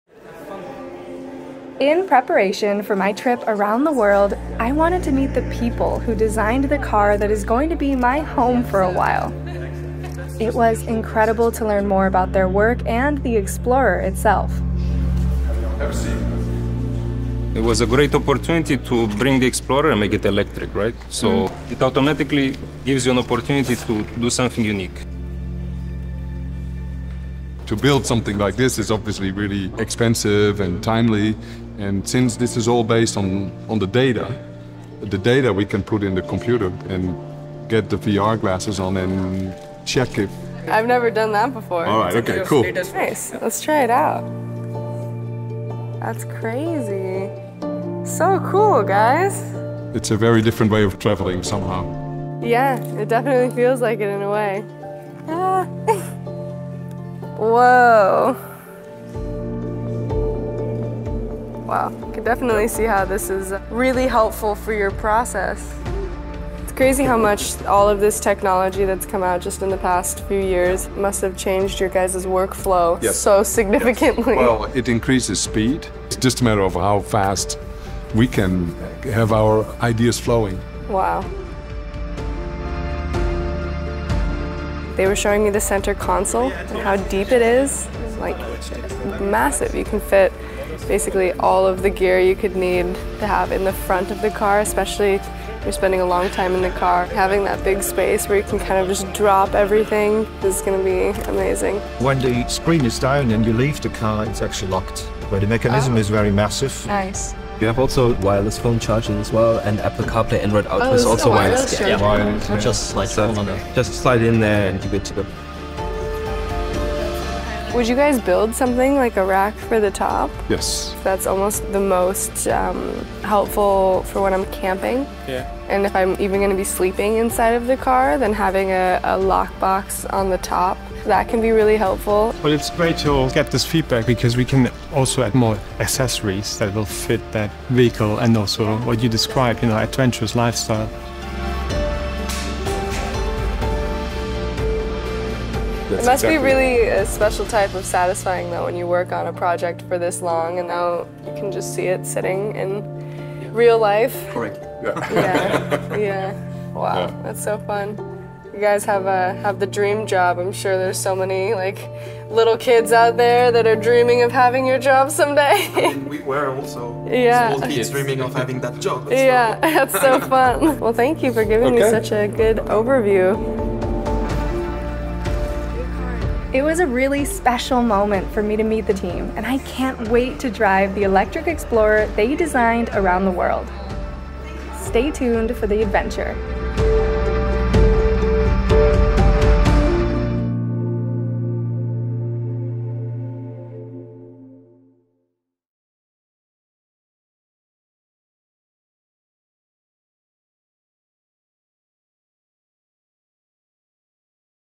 Charge Around the Globe: Lexie Meets the Team Behind Electric Ford Explorer
As she gets set for ChargeAroundTheGlobe, Lexie Limitless @LexieLimitless meets the team behind the machine.

Lexie chats to the engineers and designers that worked on the adventure-ready electric Ford Explorer.